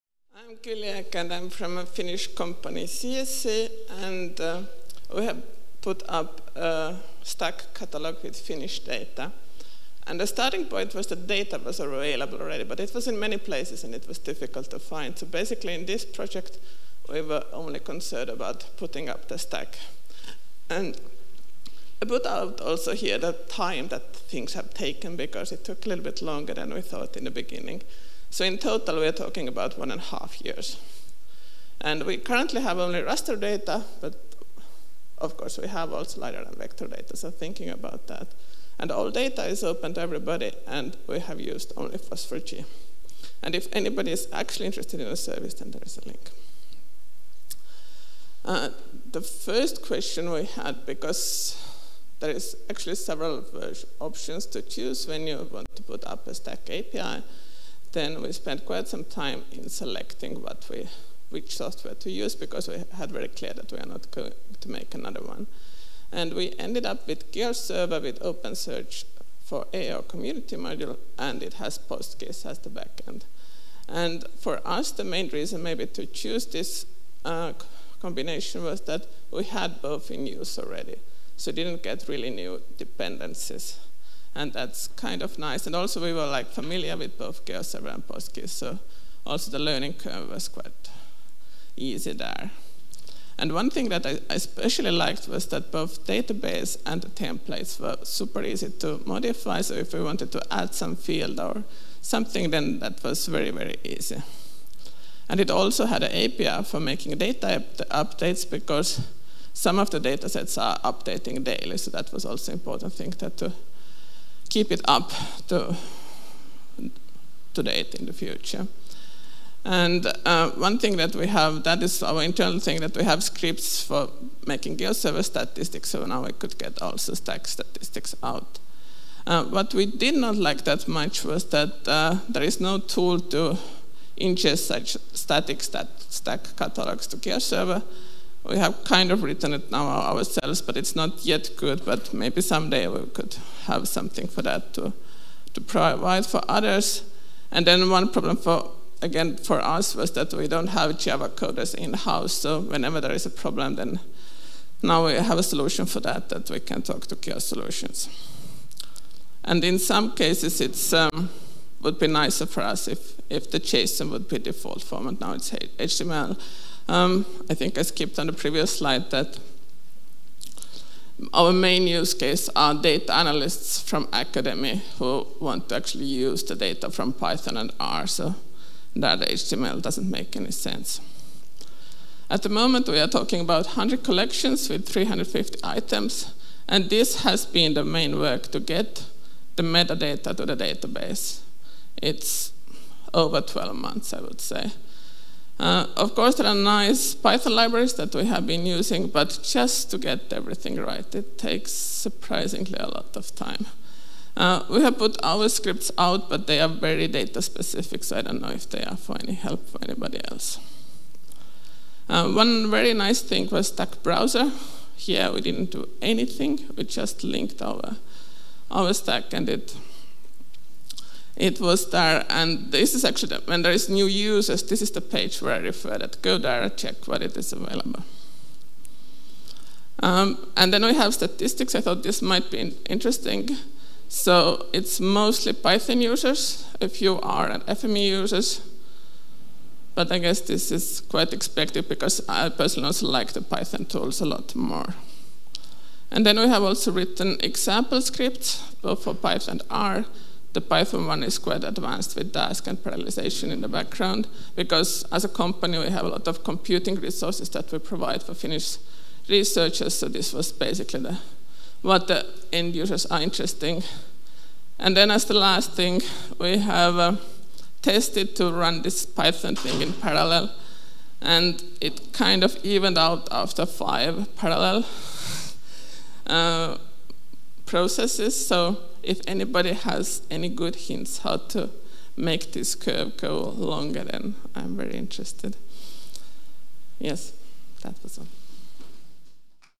FOSS4GE 2024 | Paituli STAC - experiences of setting up and using own STAC catalog
In this presentation we discuss our experiences from setting up Paituli STAC, which contains open Finnish raster datasets. We did not do any software development, but decided to use GeoServer with STAC extension. Own code was only written for populating the PostGIS database with information about ~100 collections and ~250 000 items. Paituli STAC catalog is mainly targeted for data analysis use cases, but can be used also from web applications.

We have also prepared public example scripts for using Paituli STAC with Python and R. We will also show the results of some scaling tests of using data from STAC on a supercomputer with Dask and xarray.

Külli Ek

Room: LAStools (327) @ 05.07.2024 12:20:00